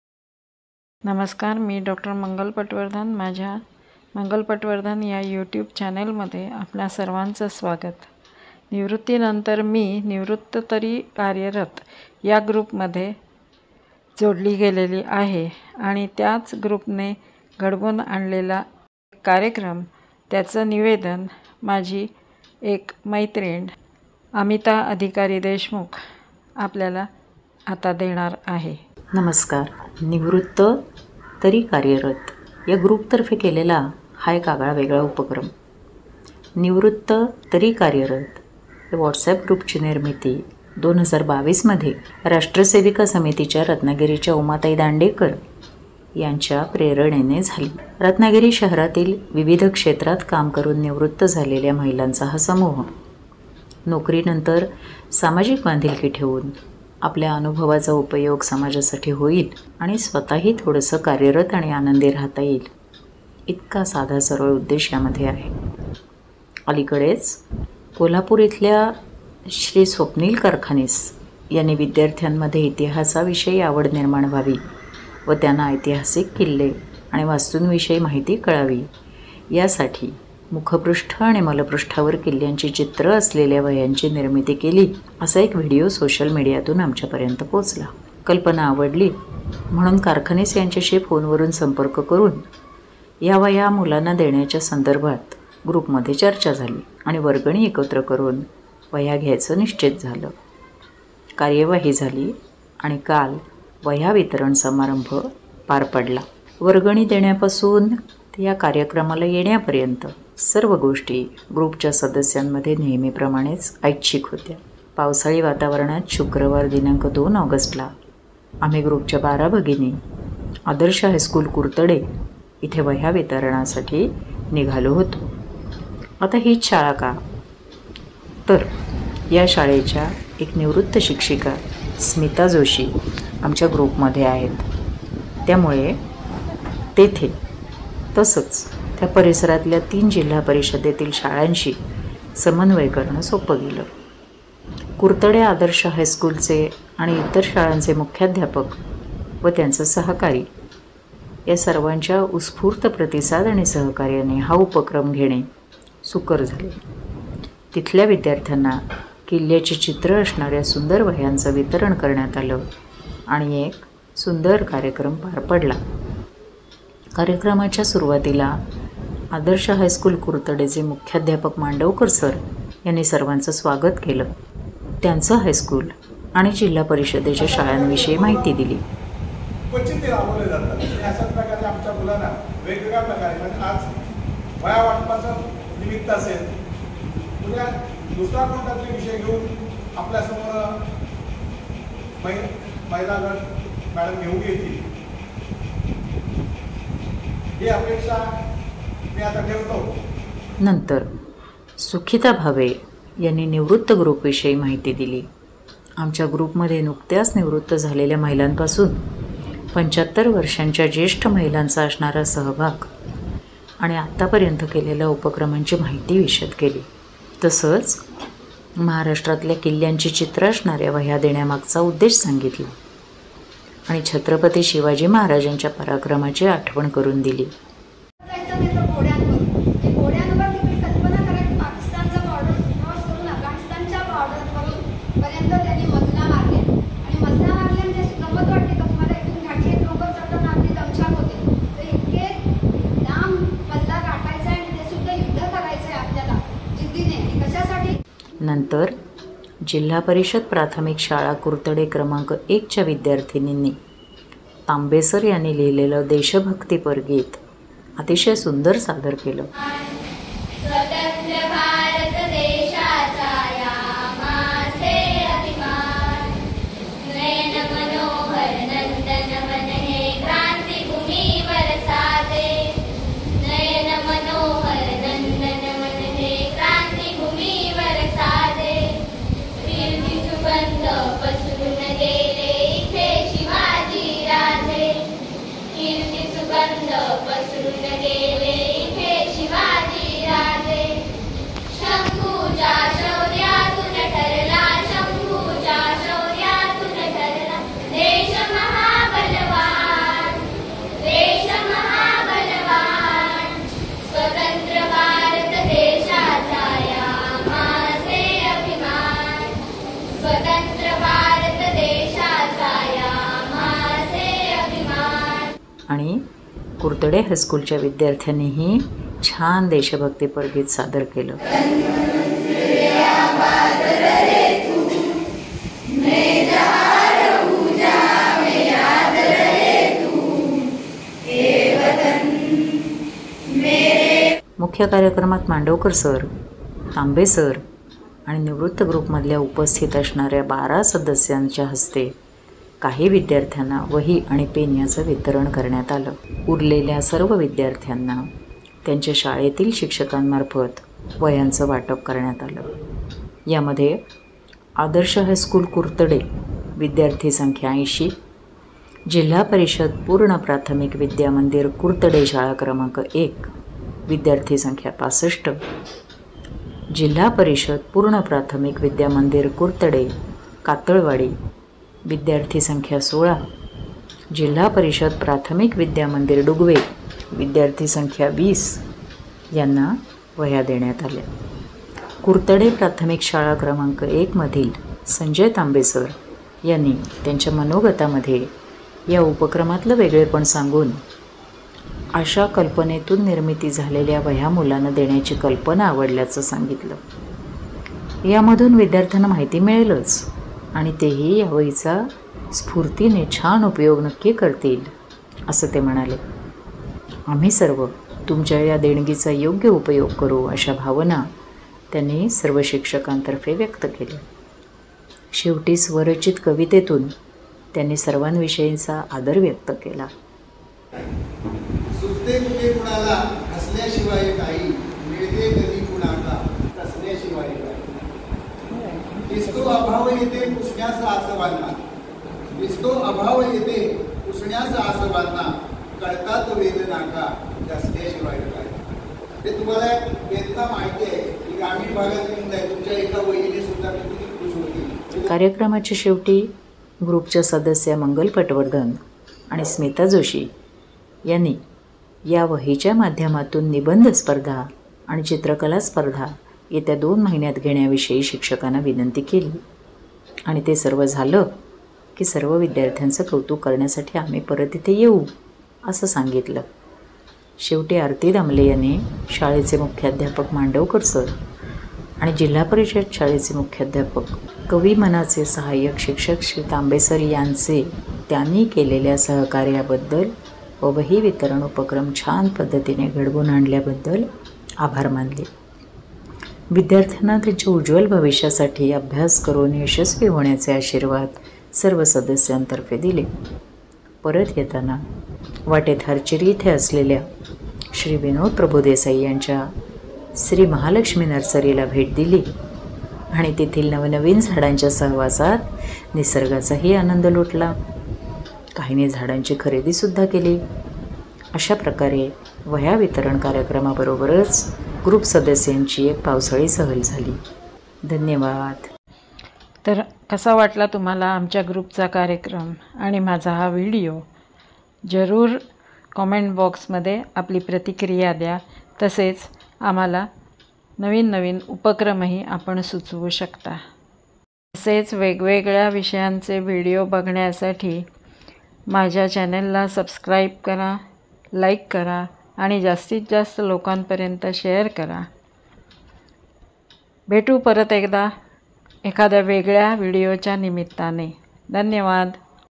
निवृत्त तरी कार्यरत ग्रुप तर्फे वह्या वितरण समारंभ
आमच्या निवृत्त तरी कार्यरत ग्रुप तर्फे श्री स्वप्नील कारखानीस यांनी विद्यार्थ्यांमध्ये इतिहासाविषयी आवड निर्माण व्हावी व त्यांना ऐतिहासिक किल्ले व वास्तूंविषयी माहिती कळावी यासाठी मुखपृष्ठ आणि मलपृष्ठावर किल्ल्यांची चित्रे असलेल्या वह्यांची निर्मिती केली त्या वह्या विकत घेऊन गरजू विद्यार्थ्यांना वितरीत केल्या. त्यावेळच्या कार्यक्रमाचा हा व्हिडिओ आहे.